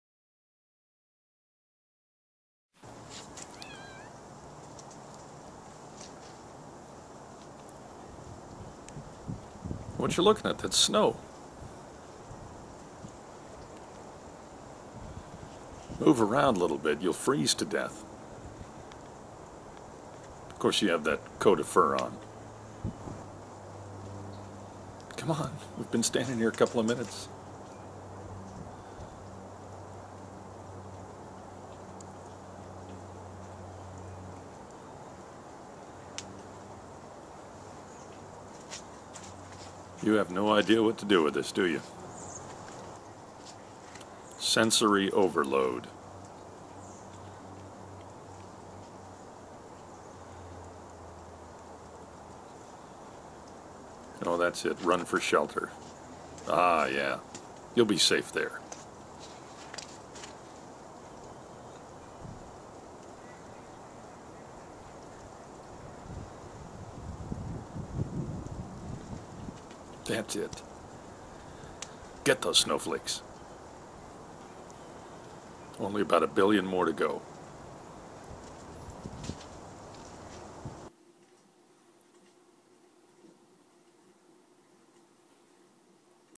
Cat's First Snowfall
Cat Diary: Grayson's first snowfall...The kitten (ha) gets his first taste of snow. He doesn't quite know what to do with it. Yet.